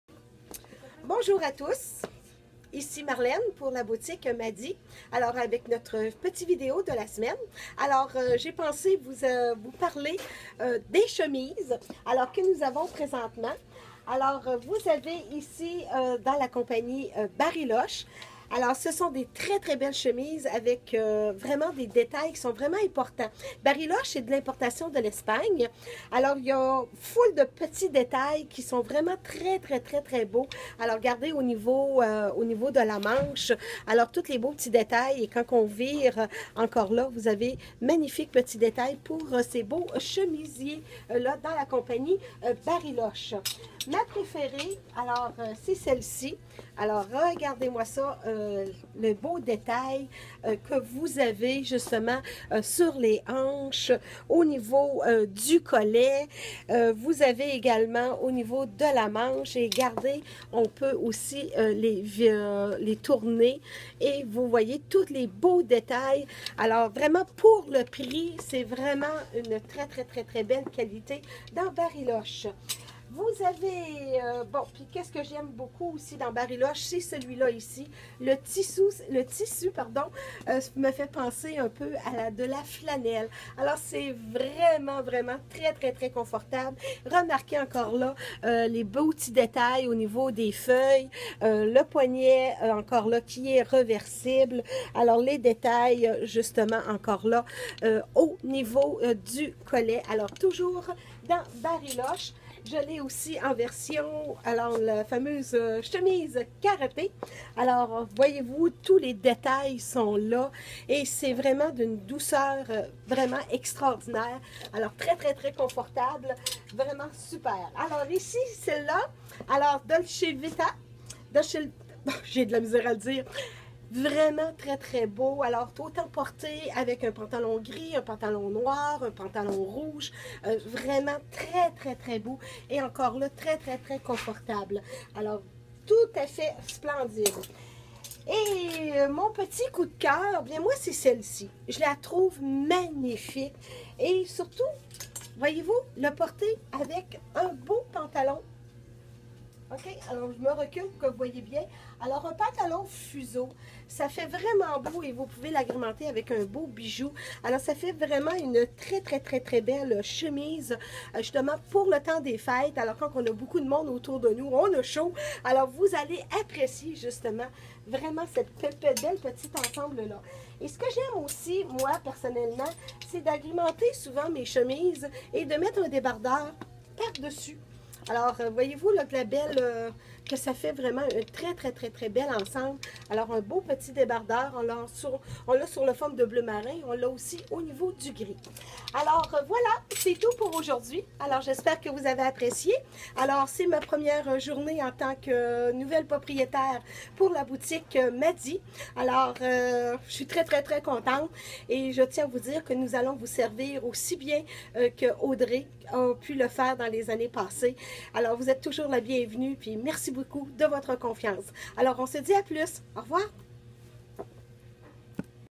Boutique Maddie - les chemisiers
Chaque semaine je vous présente les nouveautés et les tendances de la saison. Cette semaine je vous présente les chemisiers.